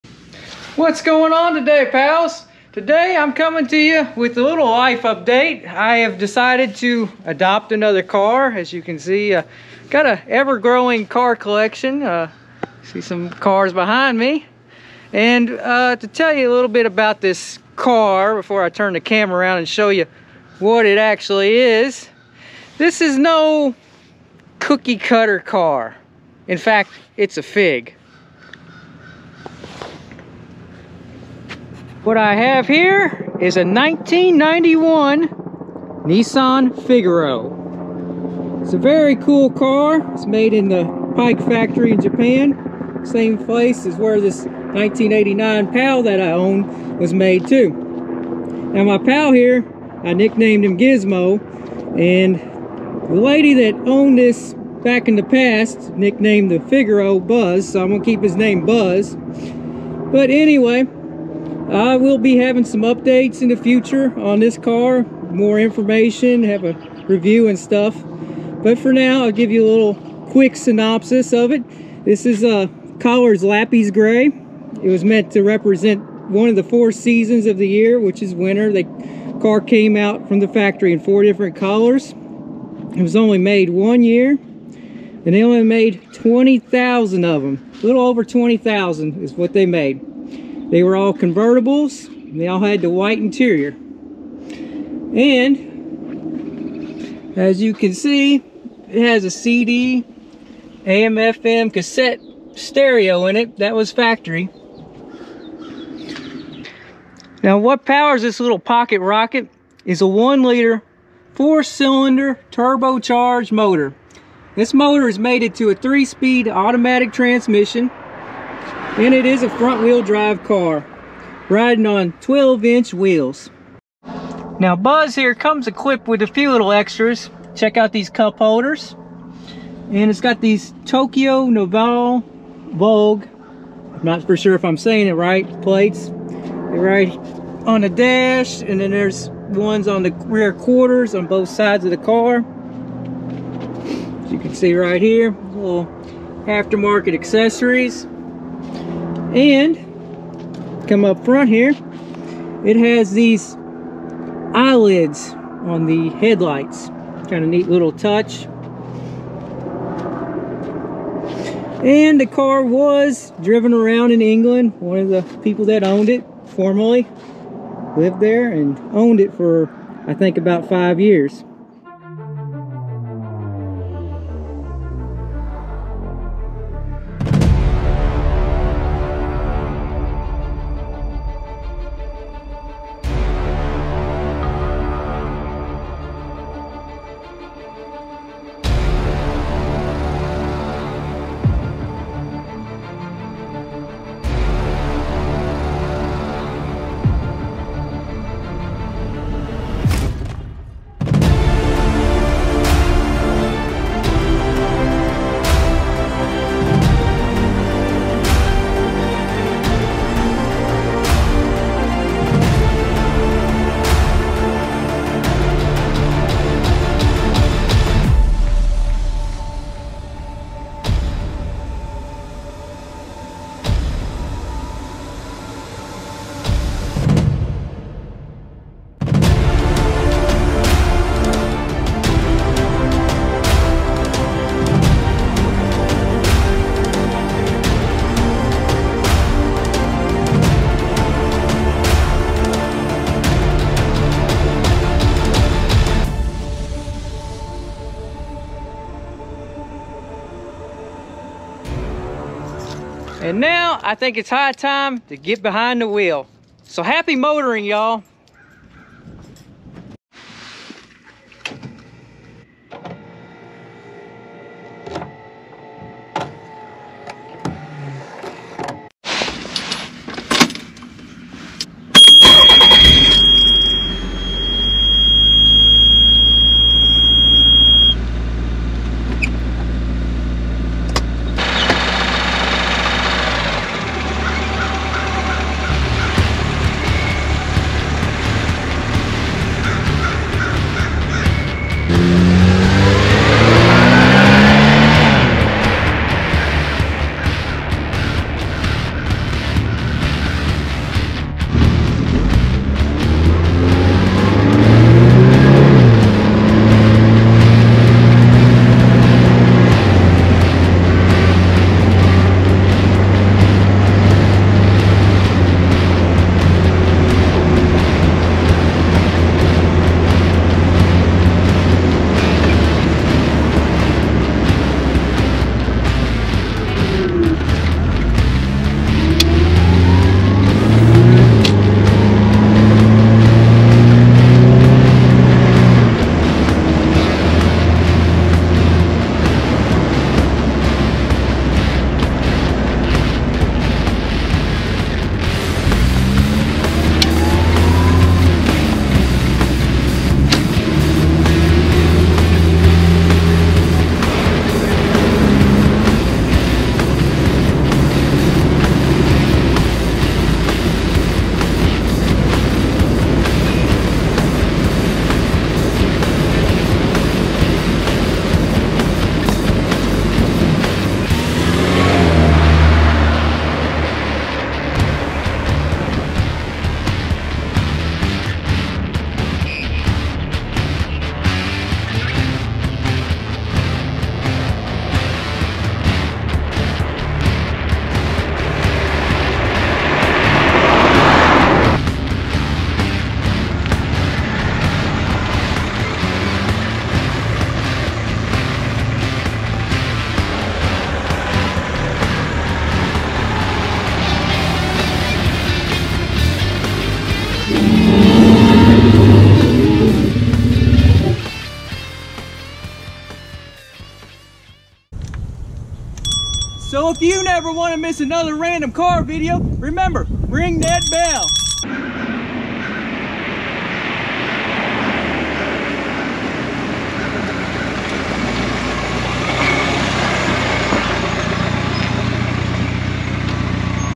LIFE UPDATE: CHECK OUT THIS LITTLE POCKET ROCKET CAR I JUST BOUGHT. WATCH TO FIND OUT WHAT CAR IT IS
Check out the video to see what cool, obscure car I just added to my collection.